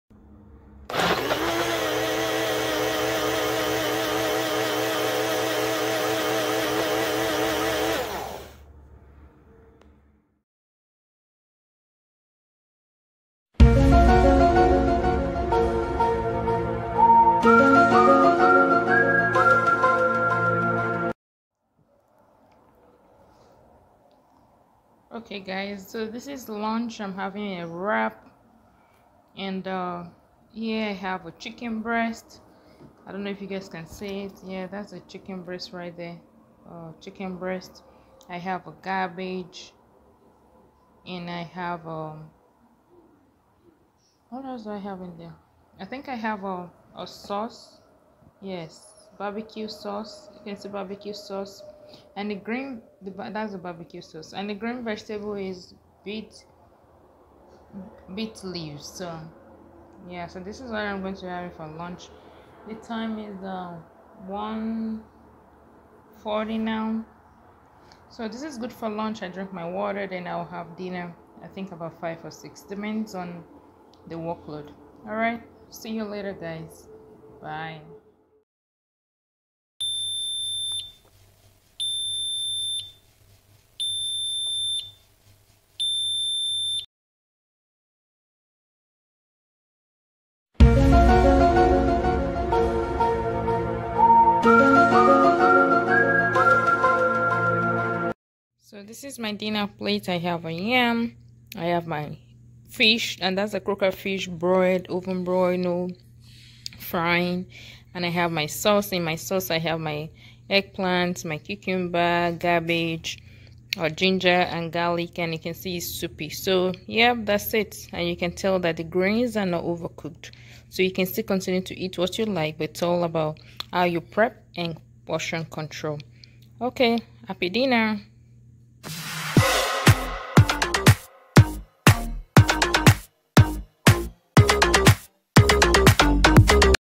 A day meal with intermittent fasting. Portion control calorie count 👌
This is to help guide meal plan, portion control, calorie count You can still eat as desire during weight loss journey. You need to be very self Discipline with portion control. Ofcourse, cheat days are allowed 3x a month or thereabout Keep pressing you are just not far from your desire healthy size